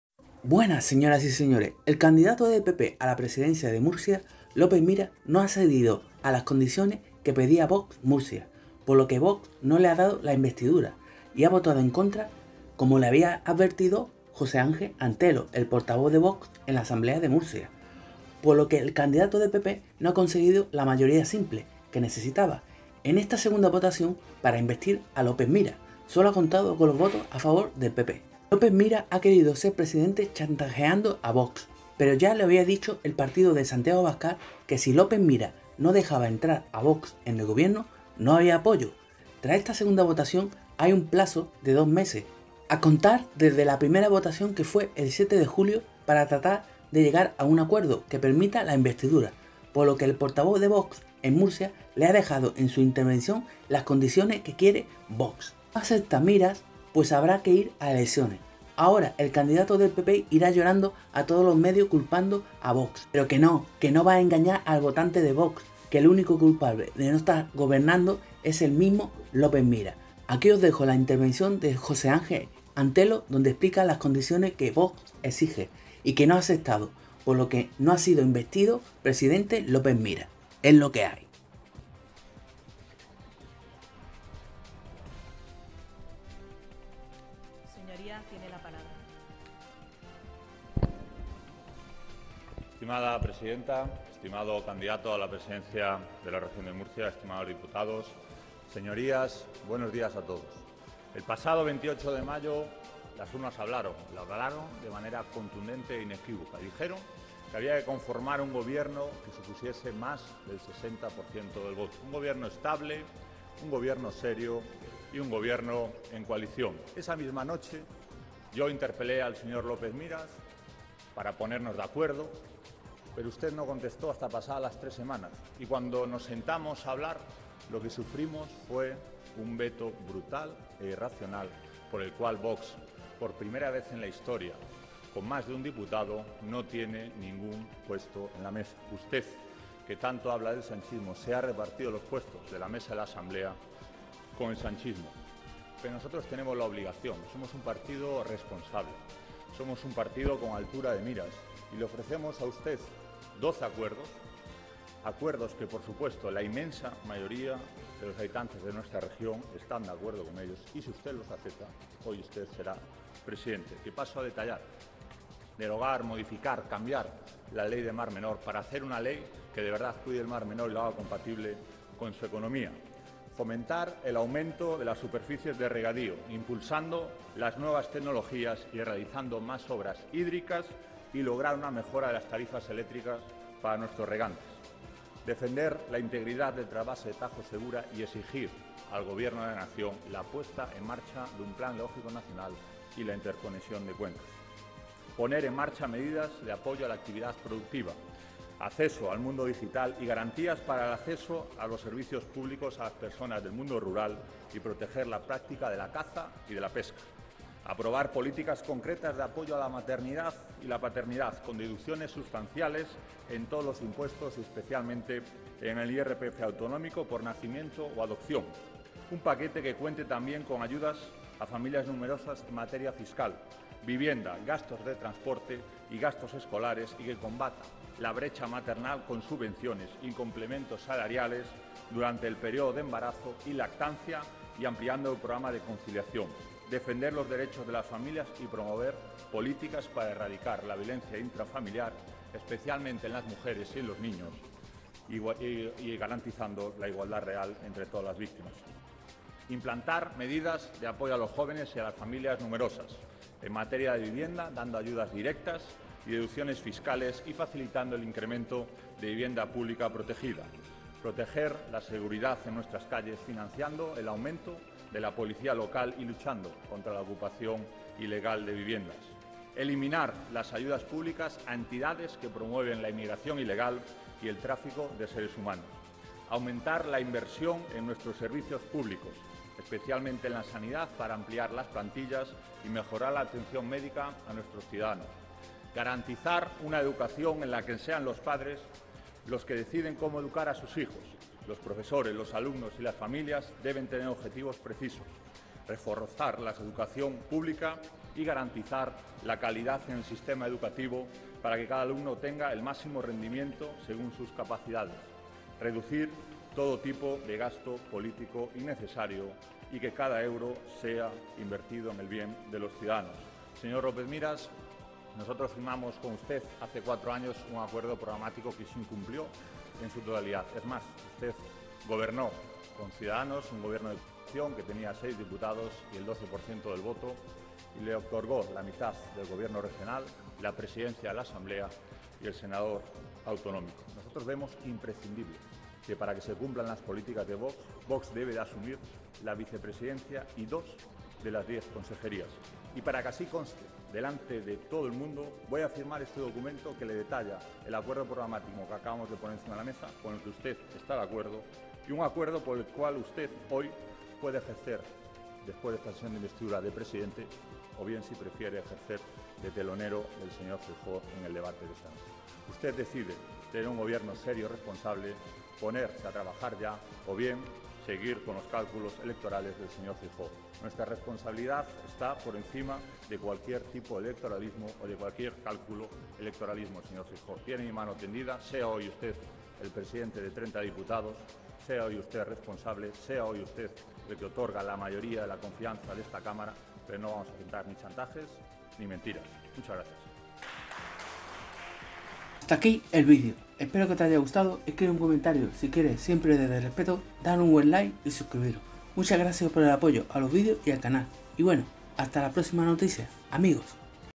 LÓPEZ MIRAS NO INVESTIDO PRESIDENTE PORQUE VOX NO LO VA HACER GRATIS
El candidato del pp de Murcia, no ha sido investido presidente de la región, porque el no ha querido aceptar las condiciones de Vox Murcia, Jose Angel Antelo se lo ha dejado claro,  no es presidente porque el no ha querido, porque no ha querido dejar entrar a vox en el gobierno